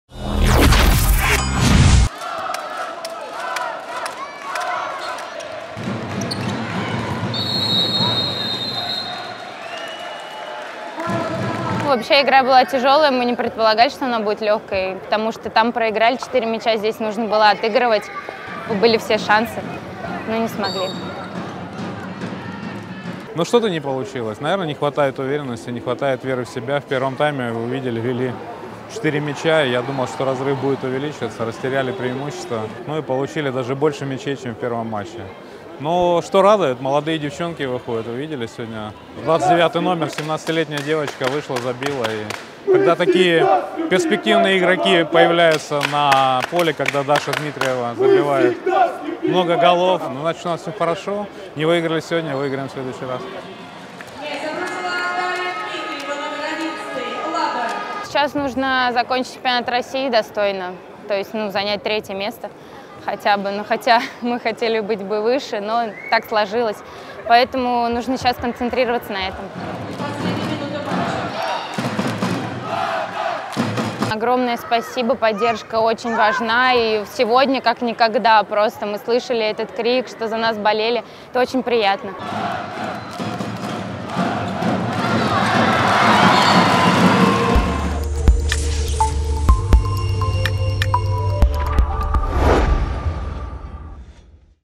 "Не выиграли сегодня, выиграем в следующий раз"
Комментарии президента ФГР Сергея Шишкарева и лидера "Лады" Дарьи Дмитриевой по итогам матча с "Тим Твис" (Дания)